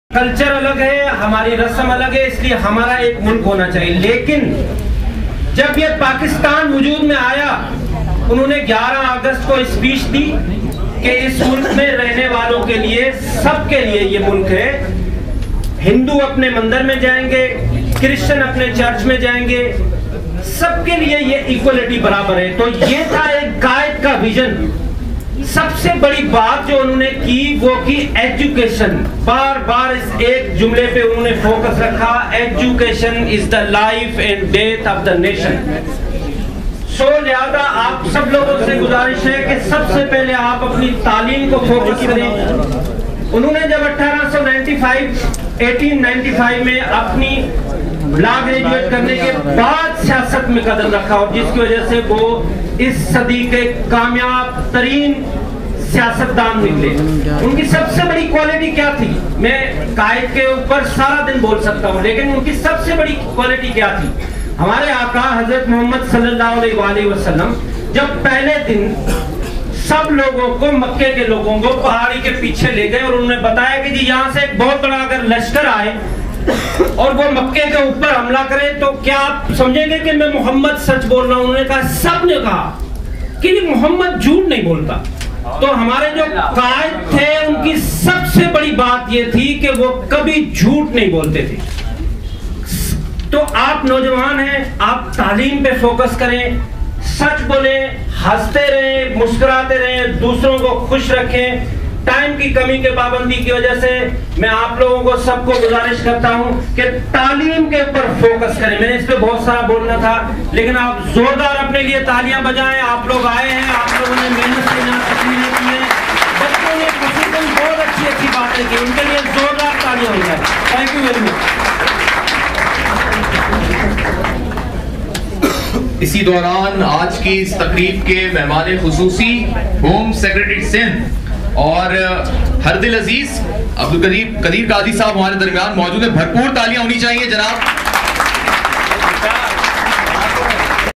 Mr. Hussain Thebo, Motivational Speaker, Senior Analyst,is addressing the YOUTH
Mr. Hussain Thebo, Motivational Speaker, Senior Analyst, Philanthropist and Youth Ambassador is addressing the YOUTH